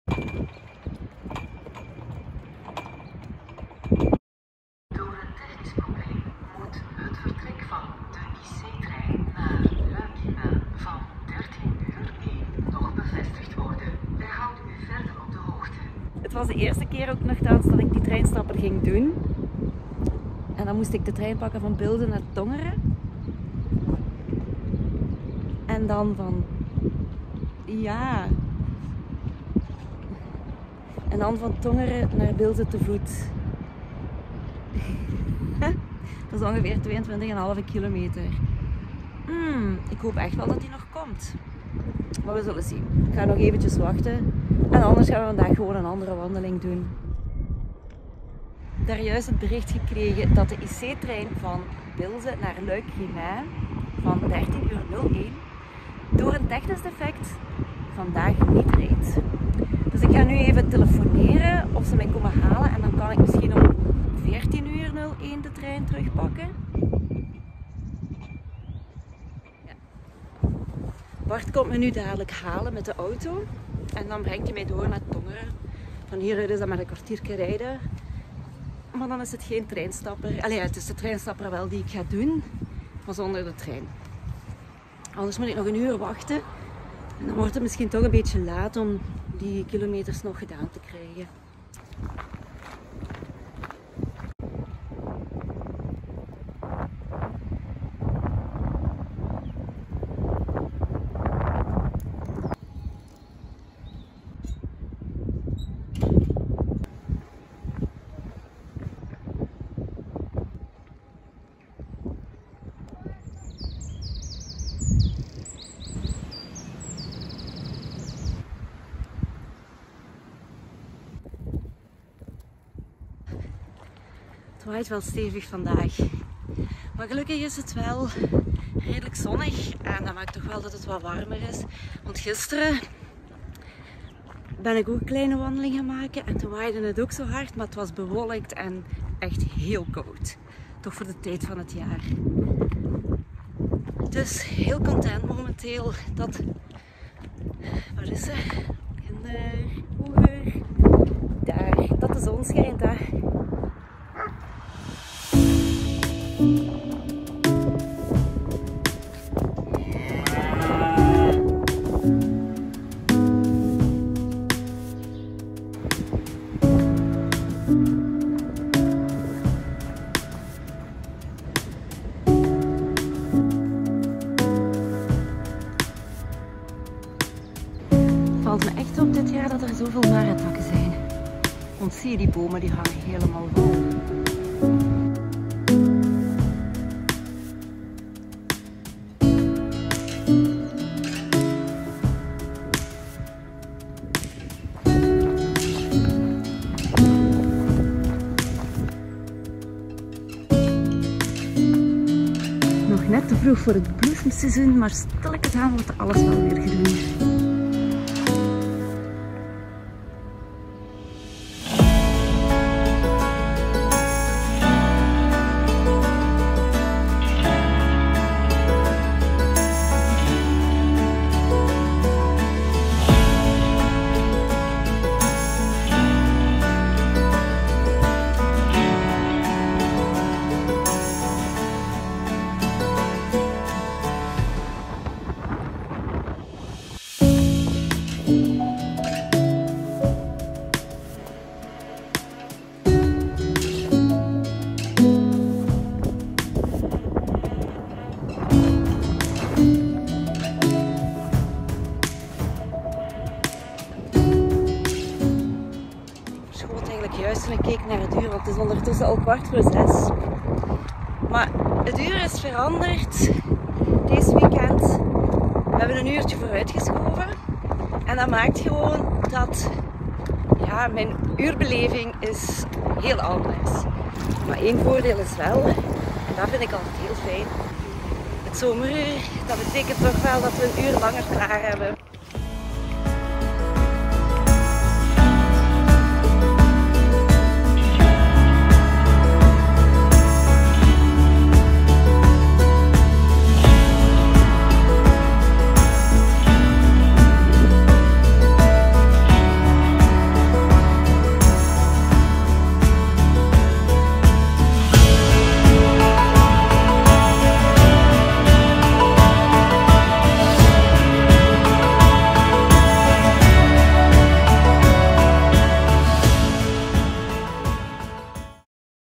Eens iets anders dan anders I Treinstapper: Tongeren Bilzen (Hiking/wandelen in Limburg)
Via de GR - grote routepaden.be kwam ik deze treinstapper tegen. Het leek mij een leuk idee om uit te proberen.
Mijn gratis gekregen railpas was nog niet veel gebruikt en dus ideaal om mijn treinrit op te noteren...
Helaas liep het even anders, maar de wandeling...nou...die heb ik wel volledig gedaan.

Music Software: Music Bed

Vond je dit filmpje leuk, laat gerust een reactie of duimpje 'vind ik leuk' achter.